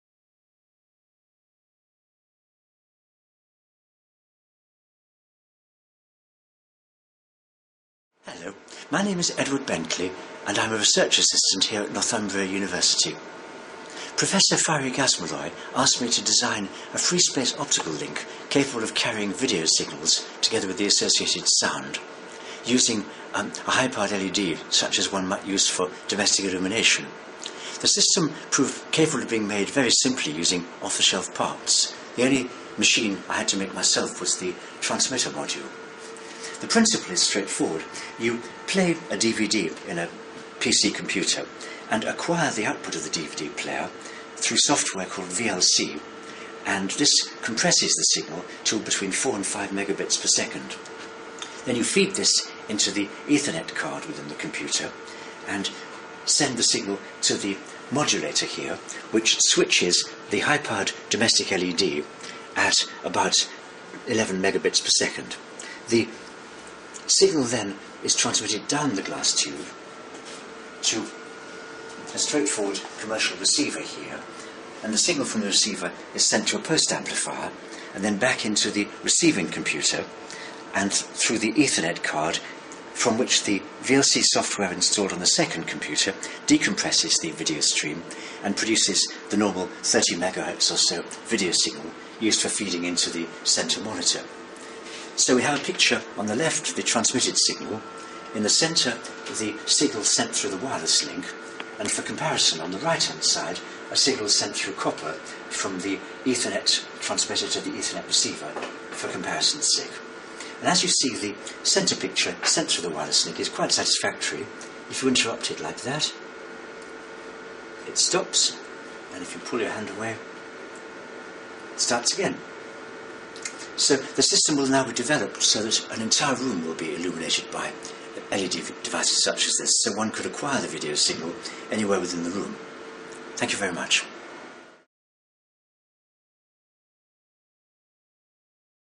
Real-Time Free-Space Optical (FSO) video transmission through VLC
Presenter: Mr. Edward Bentley -- CEIS, Research Assistant

This OCRG video describes a project to read video data within a computer using VLC software, and output the compressed data via the LAN port to a driver which modulates an illumination LED. The resulting light travels to a detector diode +amplifier, whose output is arranged to emulate a LAN signal and be processed by a second computer using VLC software to decompress the signal and drive a monitor display to play the audio+video streams.